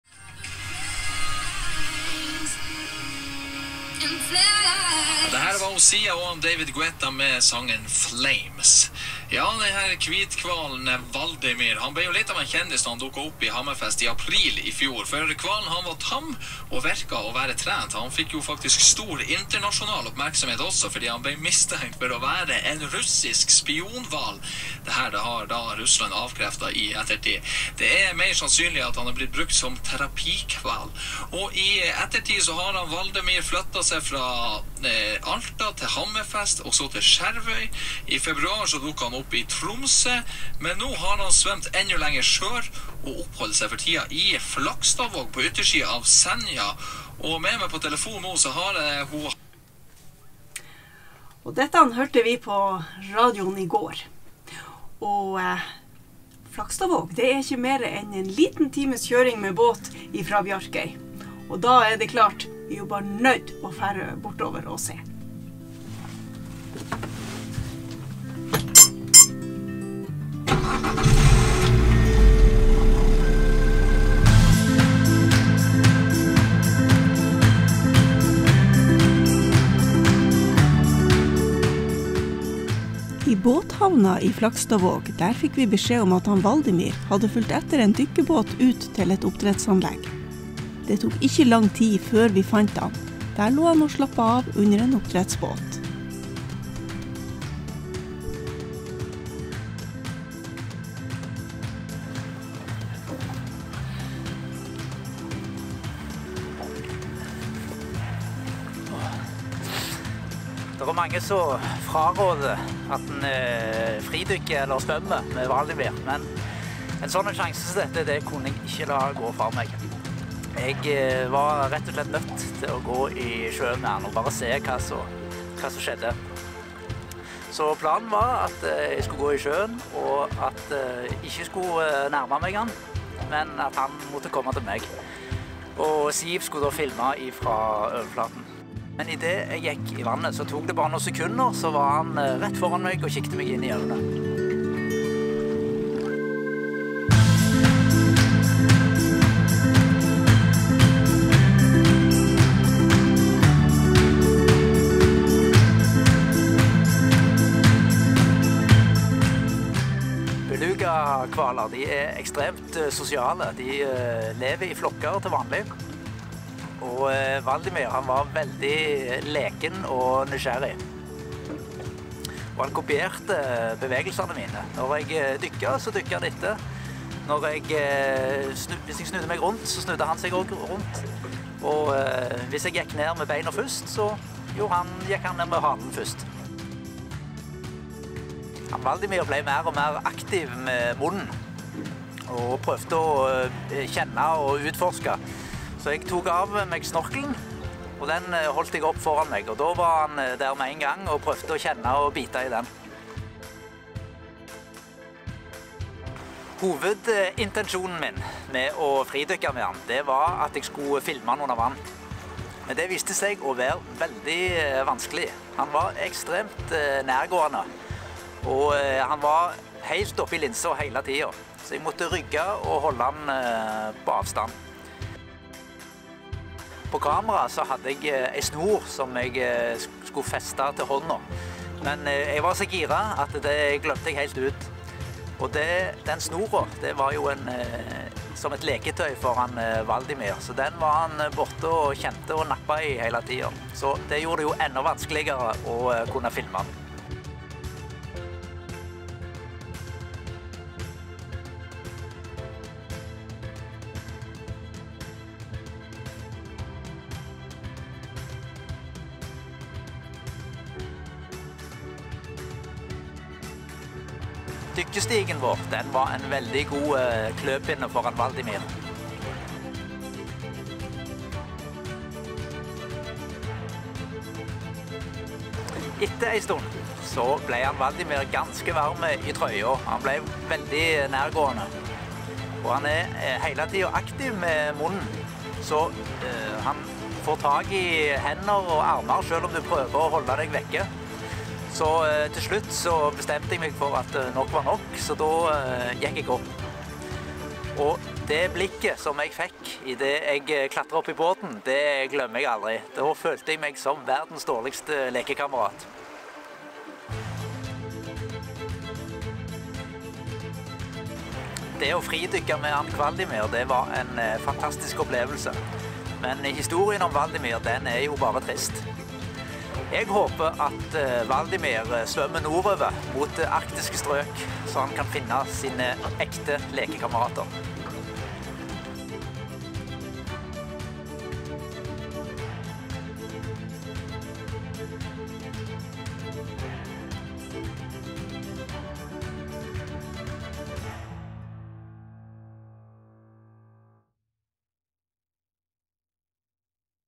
WeDive meets Hvaldimir (The runaway beluga whale)
Hvaldimir is a beluga whale which was discovered in Finnmark Northern Norway in April 2019. He was wearing a harness and appeared to be trained. Some say he is trained by the russian army, and some claim that he most likely has been used for therapy. He has wandered around Northern Norway since then and we had the opportunity to free dive with him when he was observed near Flakstadvåg, Senja.